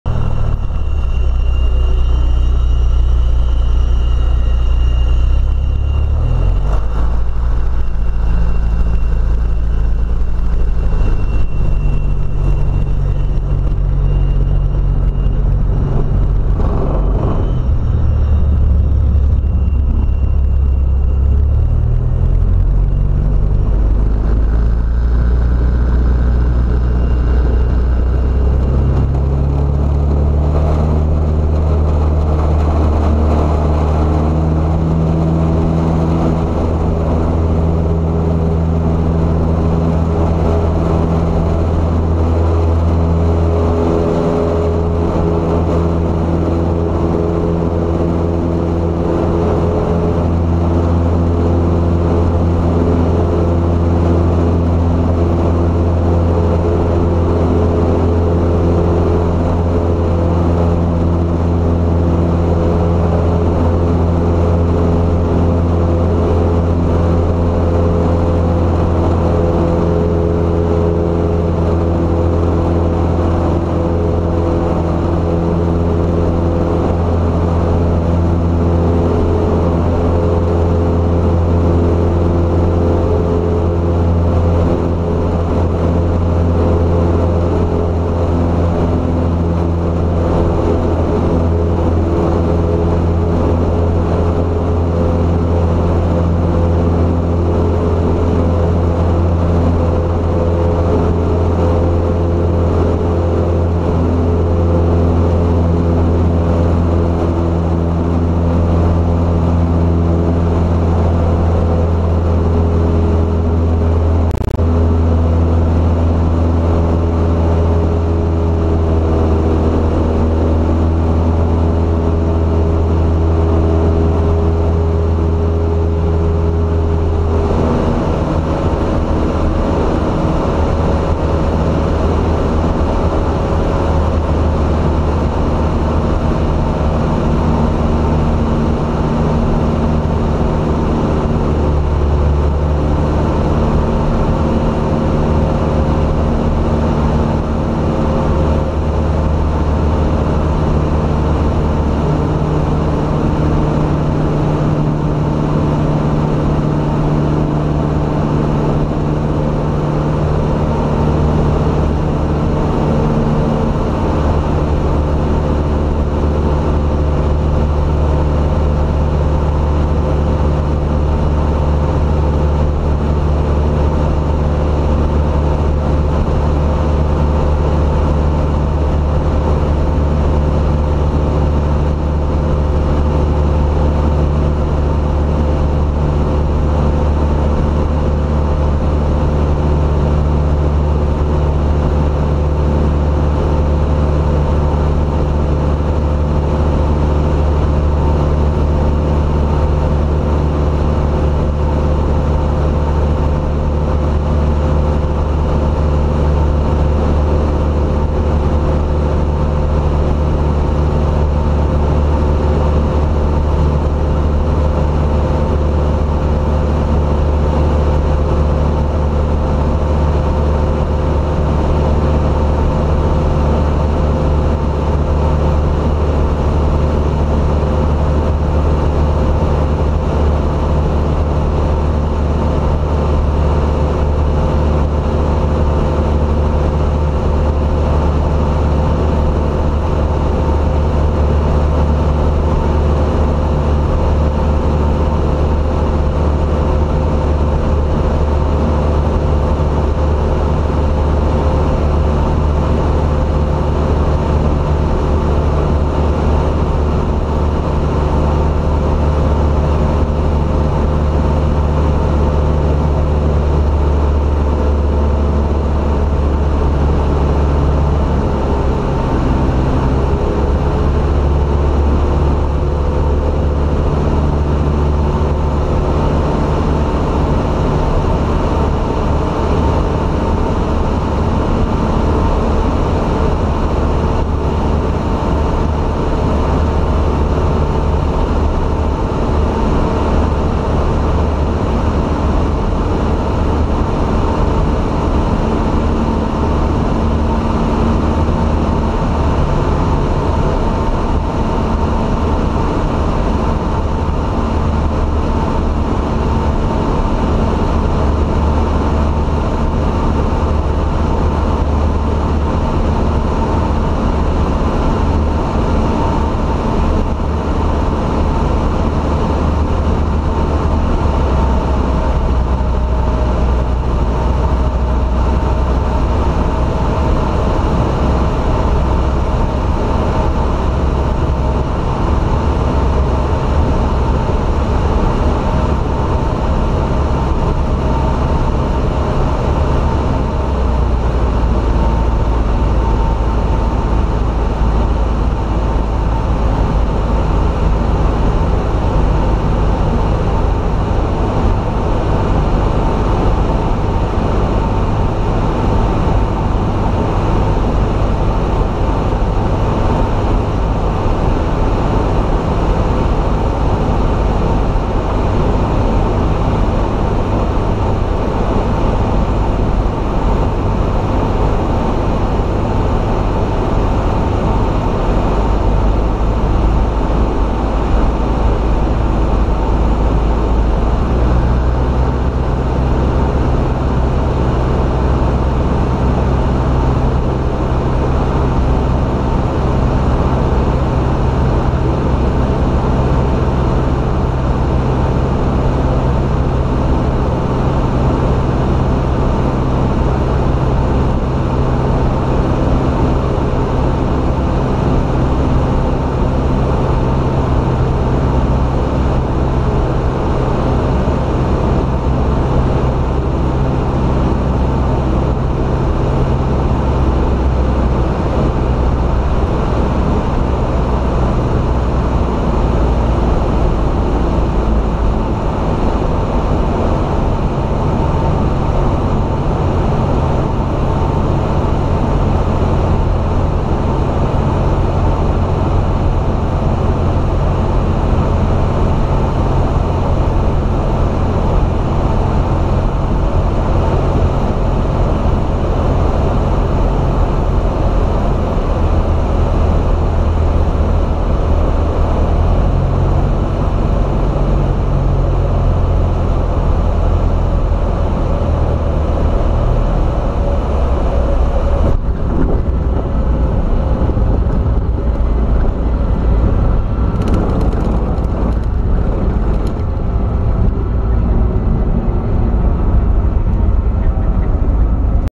My first solo flight!
After 9 days of accelerated flight training I am going solo!